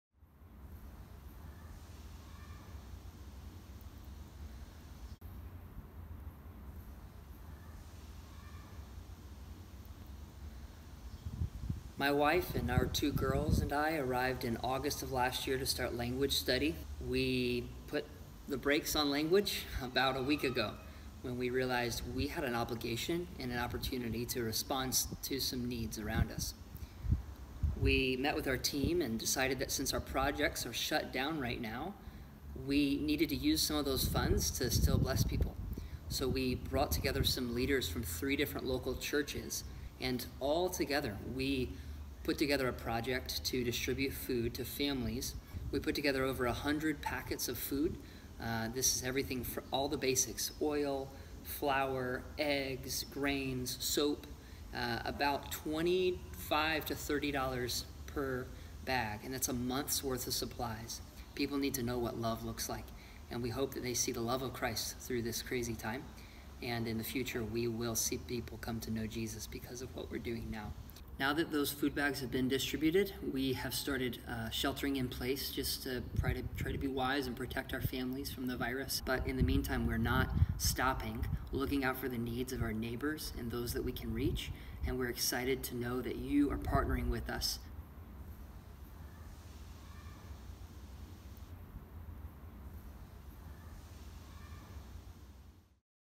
Caring in the North and Central Asia Region
An Alliance team distributed a one-month’s supply of food and soap to 100 families in their network who have been hit hard by their country’s closed borders.